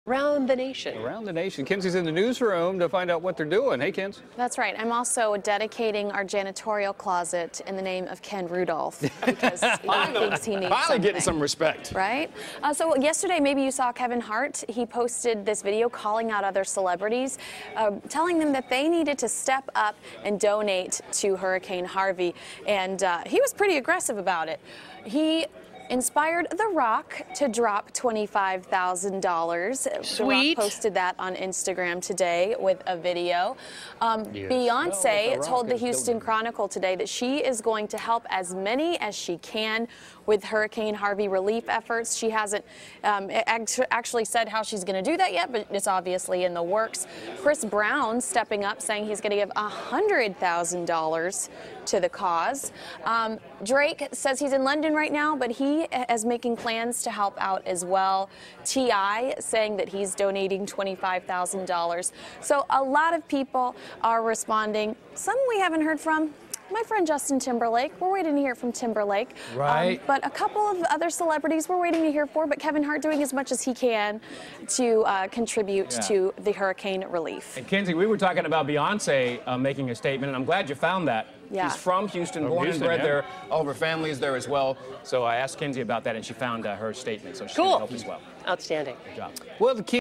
Celebrities Donating to Hurricane Harvey
Celebrities are stepping up big in the efforts to help those in Southeast Texas and Southwest Louisiana.